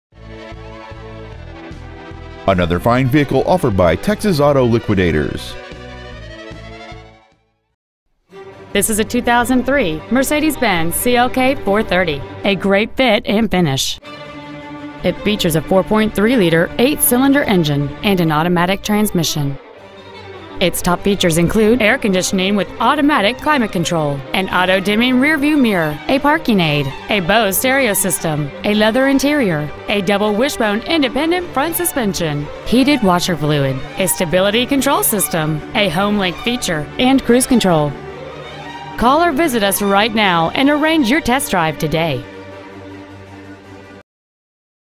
2003 Mercedes-Benz CLK430 Dallas TX
We are proud to present this 2003 Mercedes-Benz CLK430.

 Year: 2003
 Make: Mercedes-Benz
 Model: CLK430
 Engine: 4.3L SOHC SMPI 24-valve aluminum-alloy V8 engine
 Trans.: Automatic
 Exterior: Silver
 Miles: 131,379
 Interior: Gray

 Preowned 2003 Mercedes-Benz CLK430 Arlington TX